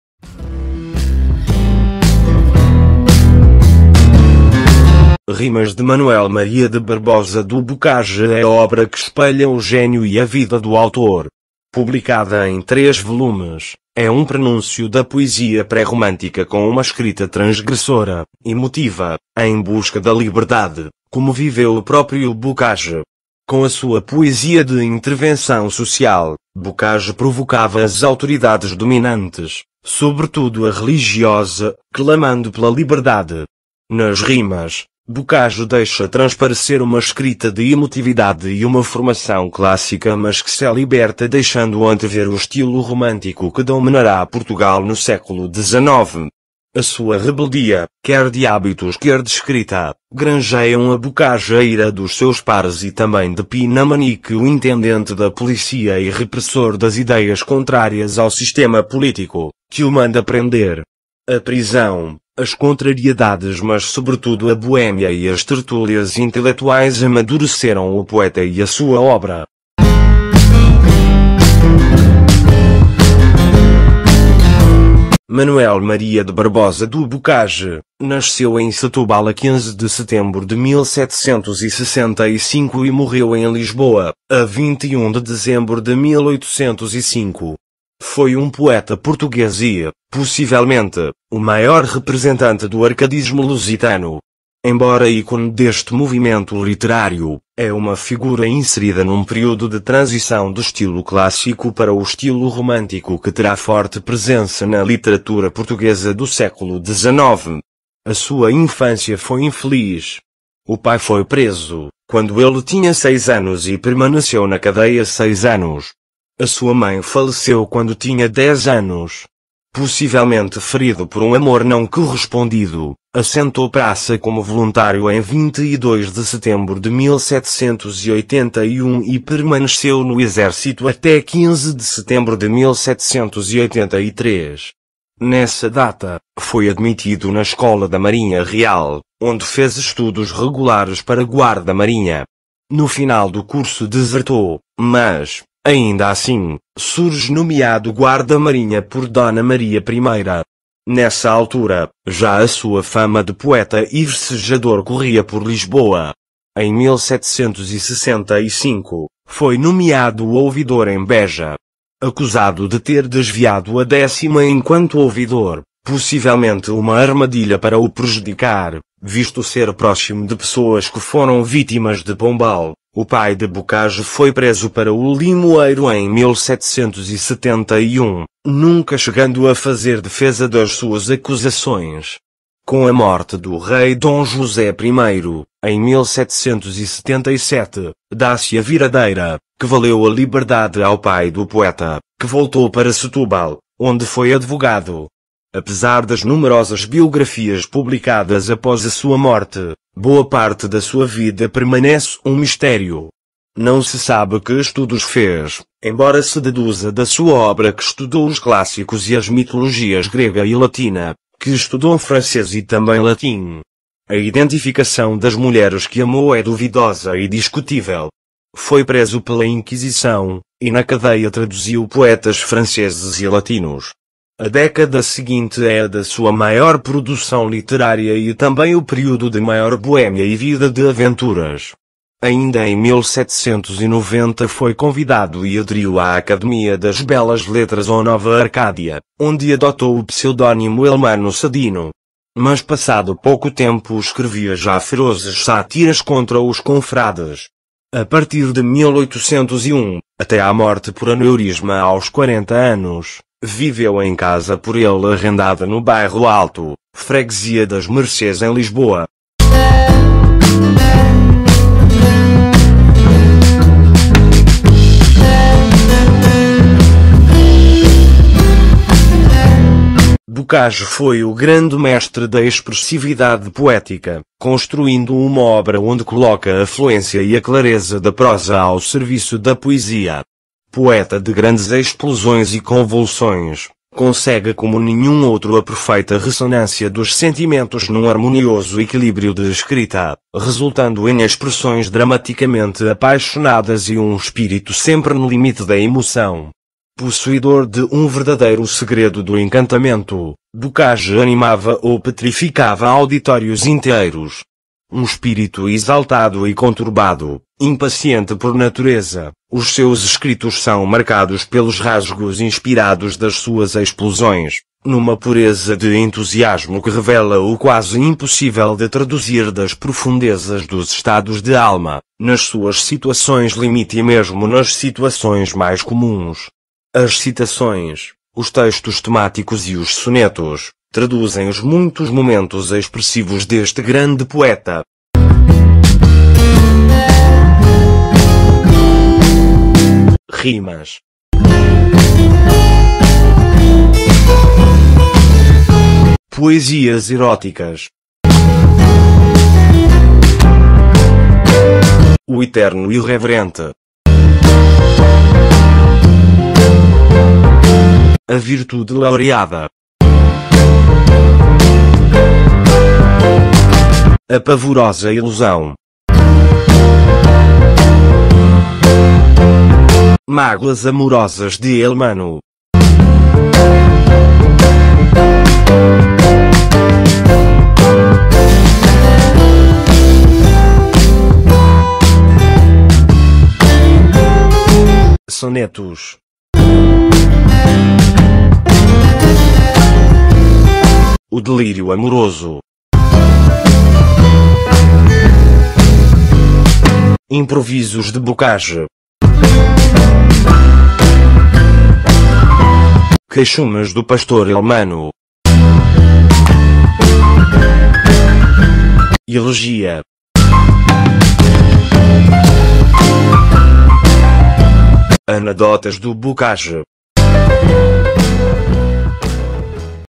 MANUEL MARIA DE BARBOSA DU BOCAGE
"Rimas" by Manuel Maria de Barbosa du Bocage is the work that mirrors the genius and life of the author. Published in three volumes, it is a harbinger of pre-romantic poetry with a transgressive, emotive writing, in search of freedom, as Bocage himself lived. With his poetry of social intervention, Bocage provoked the dominant authorities, especially the religious, calling for freedom. In the "Rimas", Bocage reveals a writing of emotionality and a classic formation but that is released leaving anticipating the romantic style that will dominate Portugal in the XIX century. His rebelliousness, both in habits and in writing, earned Bocage the wrath of his peers and also of Pina Manique the Intendant of the police and repressor of ideas contrary to the political system, which orders him to arrest. The prison, the setbacks but above all bohemia and the intellectual tertulias matured the poet and his work.